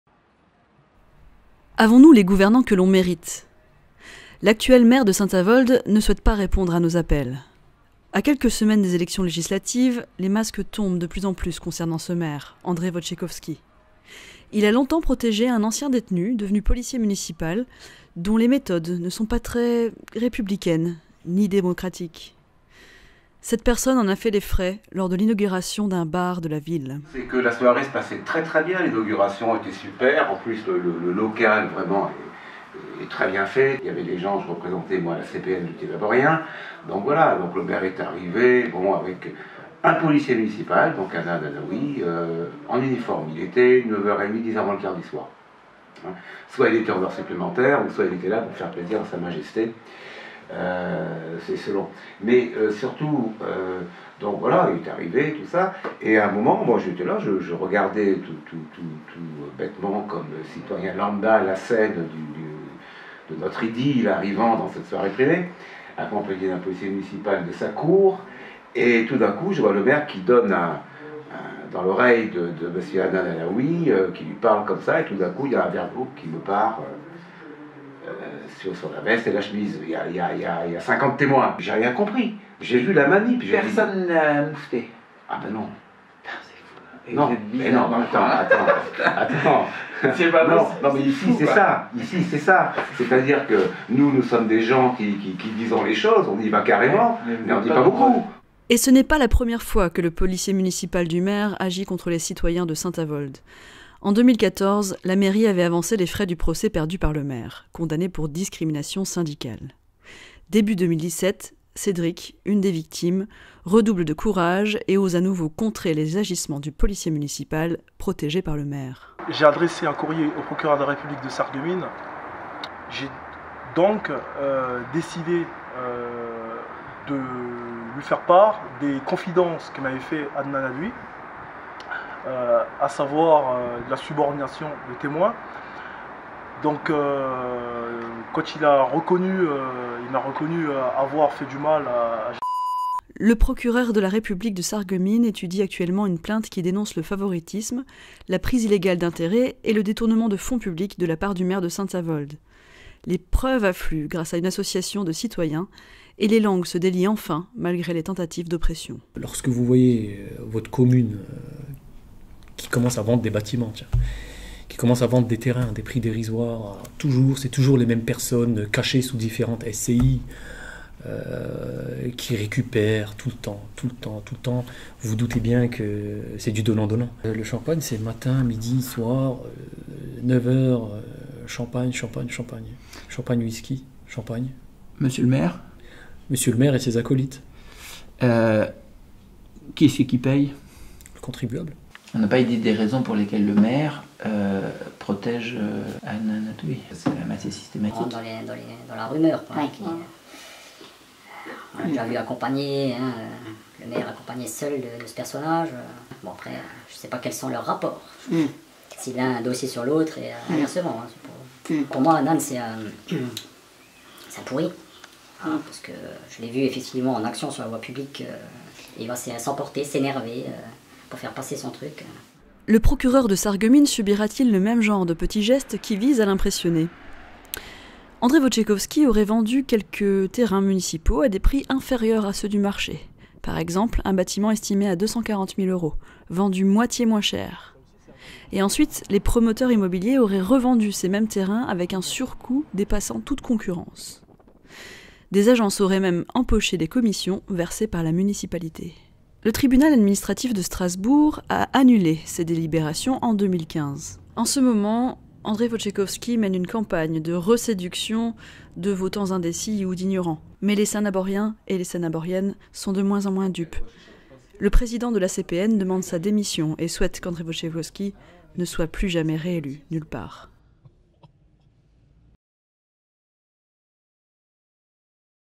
Maire ou voyou ? nouveau cas à Saint-Avold
Une association d'habitant.e.s de St-Avold dépose plainte contre le maire de Saint-Avold (depuis 2001) : M.André Wojciechowski